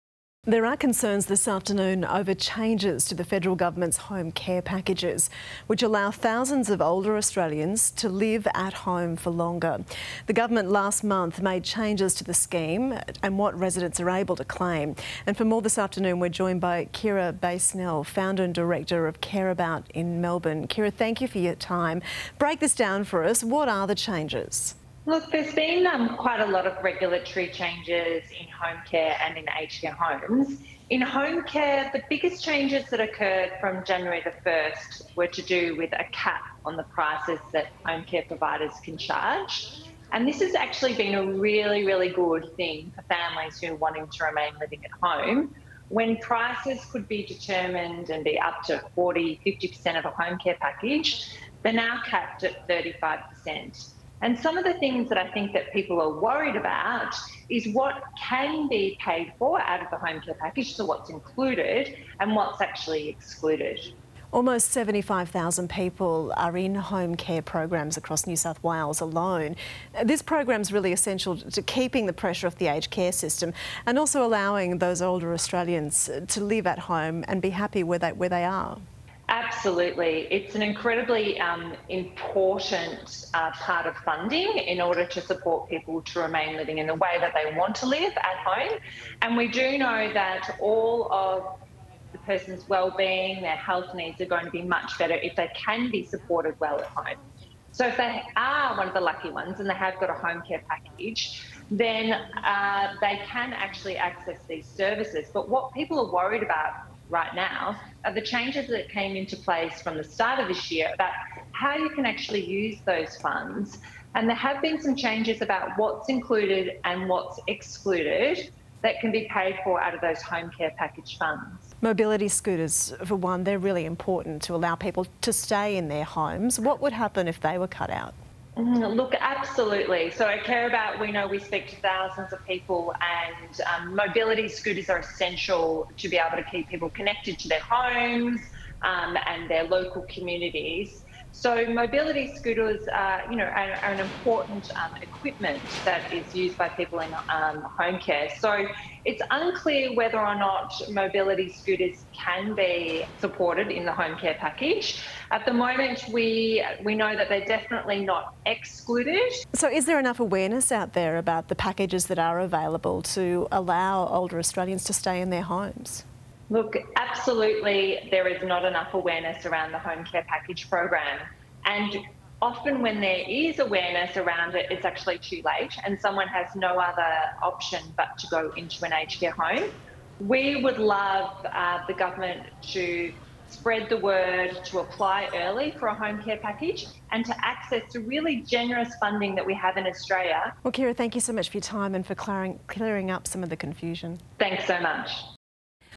HOME CARE CHANGES 2023 CareAbout@9NewsAUS
On 14th February 2023 CareAbout Co-Founder Kyra spoke with @9NewsAUS about how Home Care Packages provides affordable care services for the elderly and the new changes to the Home Care guidelines.